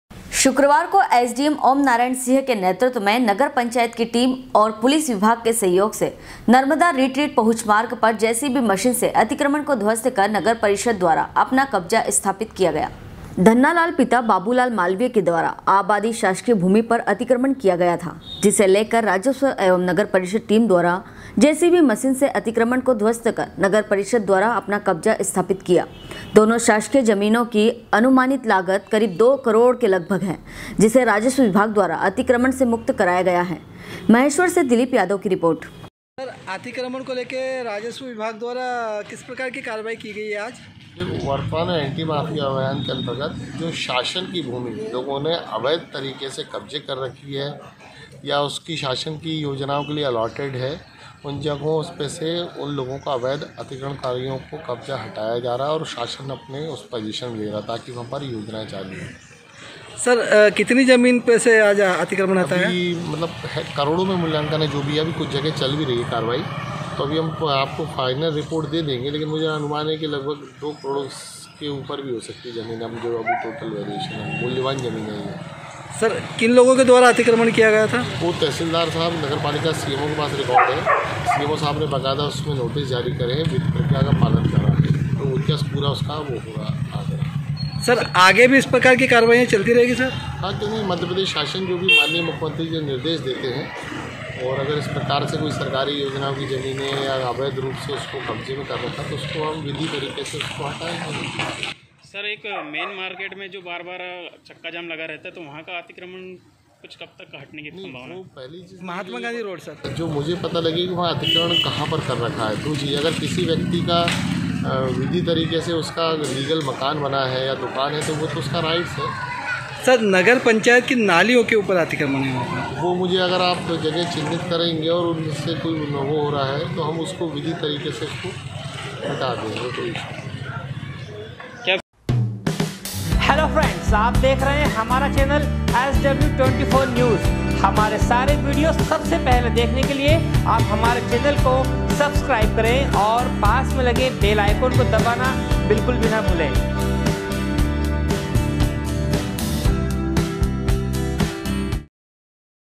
SW 24 News Madhyapradesh
शुक्रवार को एसडीएम ओम नारायण सिंह के नेतृत्व में नगर पंचायत की टीम एवं पुलिस विभाग के सहयोग से नर्मदा रिट्रीट पहुंच मार्ग पर जेसीबी मशीन से अतिक्रमण को ध्वस्त कर नगर परिषद द्वारा अपना कब्जा स्थापित किया। महेश्वर से दिलीप यादव की रिपोर्ट